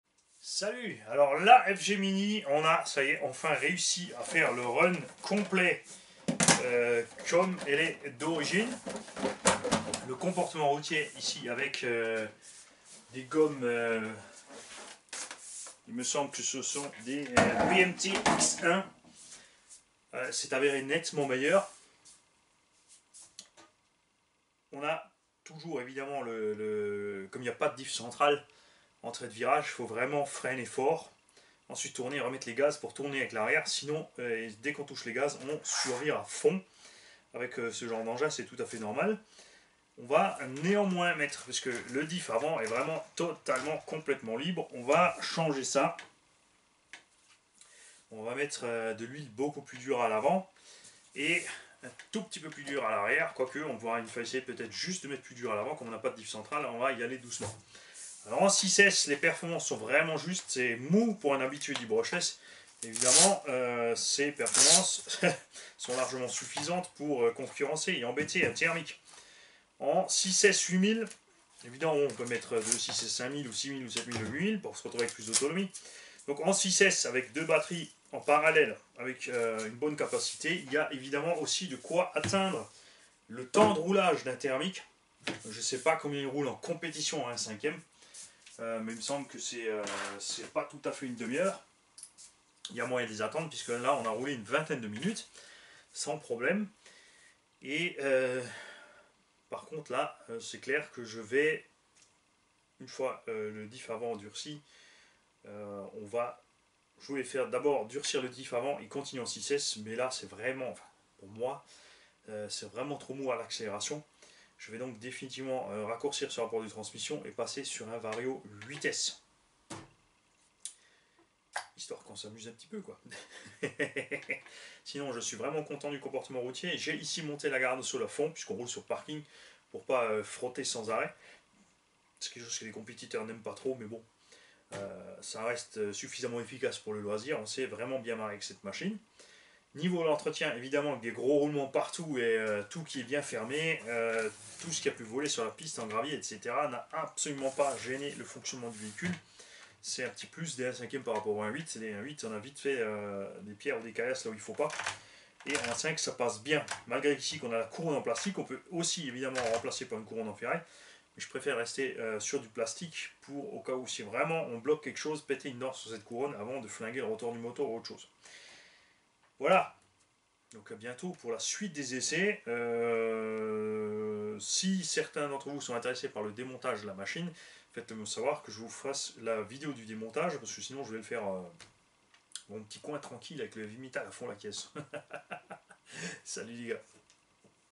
LA FG MINI BRUSHLESS 6s assure mais 8s sera mieux hiihihi
des bon pneus et hop tt va bien ! vive le 8s pour un peut + de punch
pi un pti peut de boulot sur la carro :)